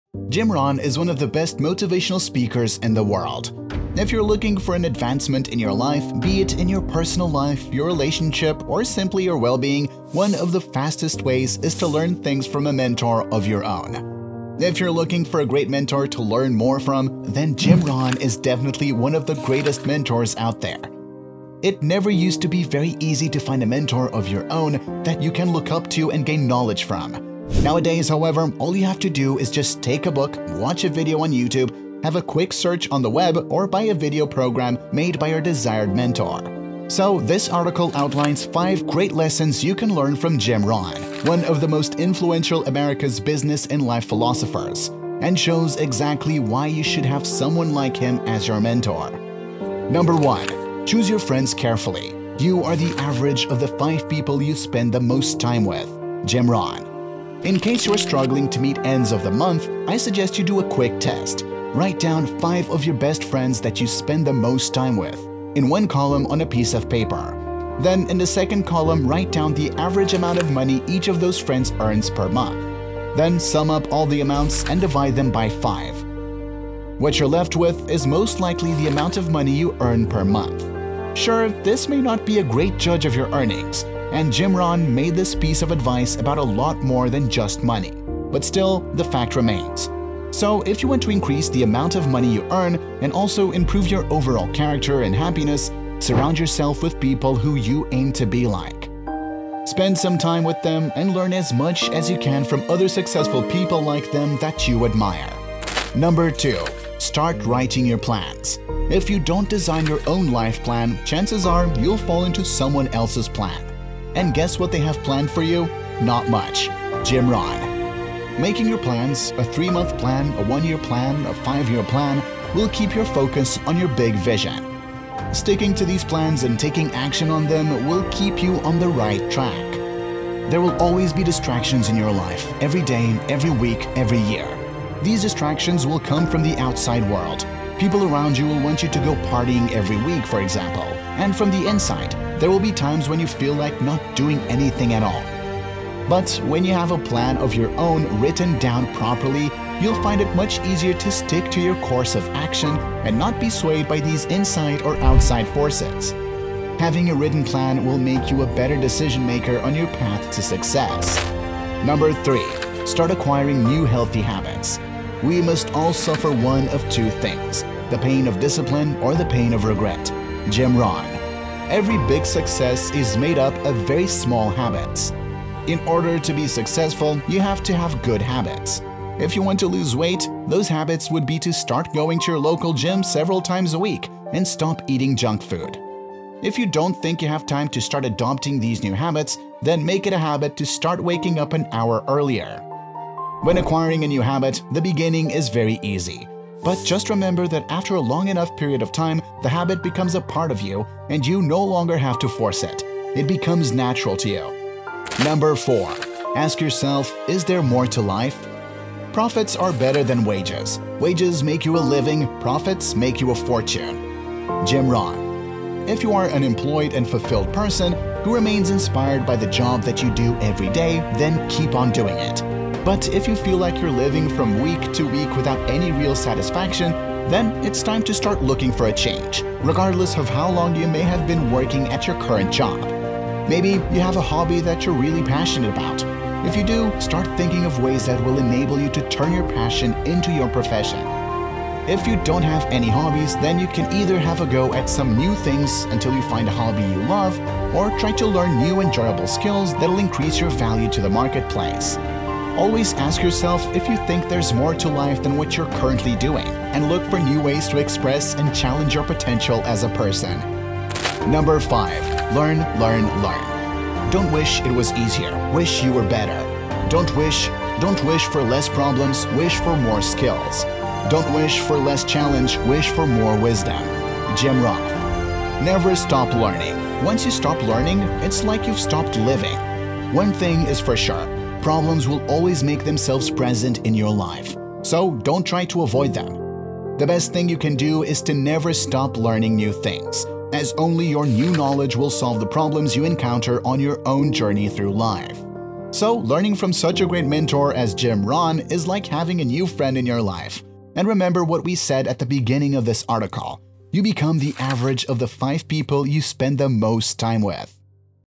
Great SUCCESS Lessons We Can Learn From JIM ROHN
Jim Rohn is one of the best motivational speakers in the world;
If you’re looking for an advancement in your life (be it in your professional life, your relationship or simply your well-being) one of the fastest ways is to learn things from a mentor of your own.
If you’re looking for a great mentor to learn more from, then Jim Rohn is definitely one of the greatest mentors out there.
It never used to be very easy to find a mentor of your own that you can look up to and gain knowledge from. Nowadays however, all you have to do is just take a book, watch a video on YouTube, have a quick search on the web or buy a video program made by your desired mentor.
So, this article outlines 5 great lessons you can learn from Jim Rohn, one of the most influential America’s business and life philosophers, and shows exactly why you should have someone like him as your mentor.